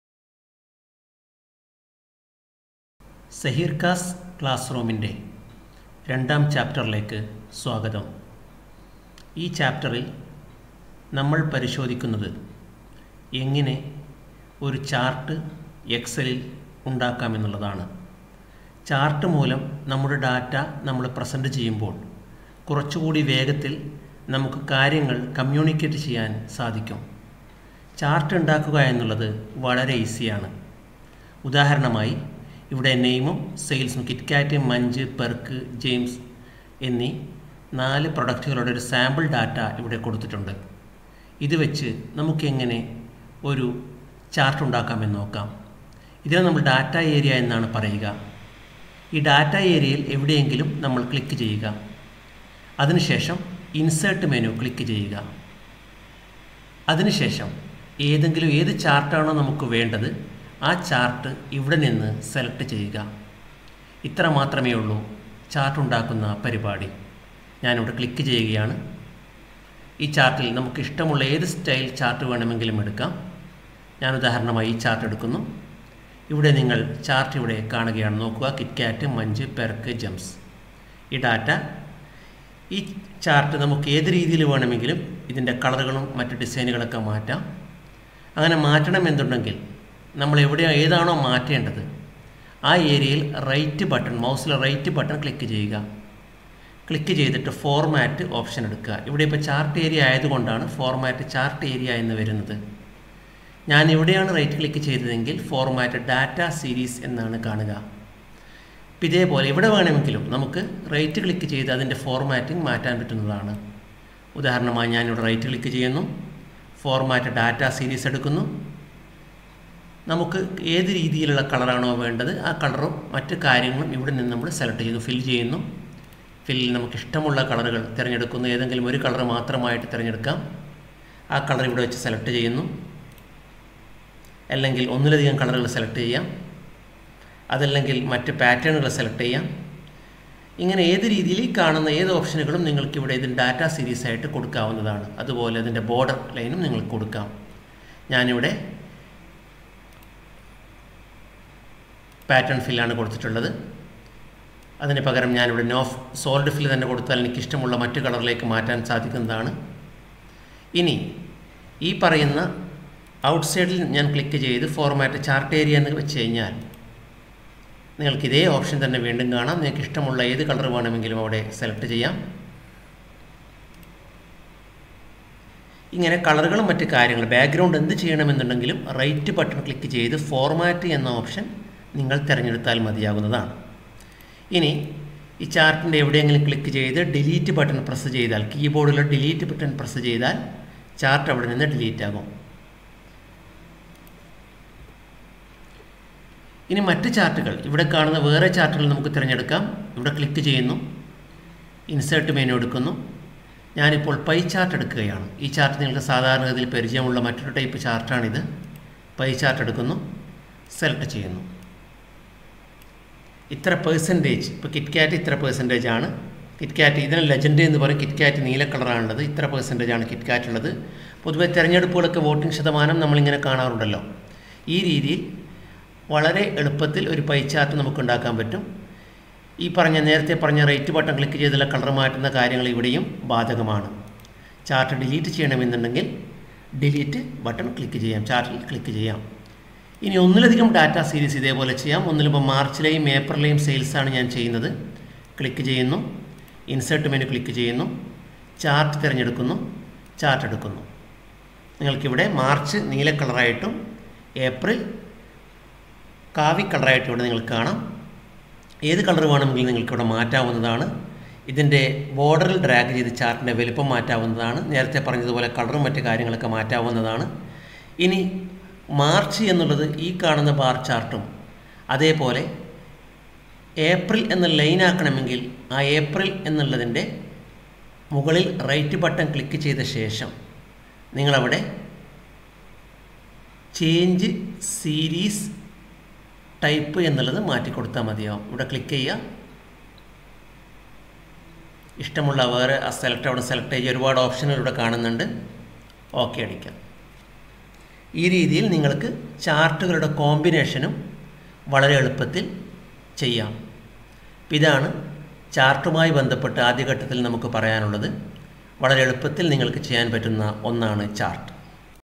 Chart - Excel Malayalam Tutorial Chapter 2
Content

1 Create Chart
2 Customize Chart
3 Combination Chart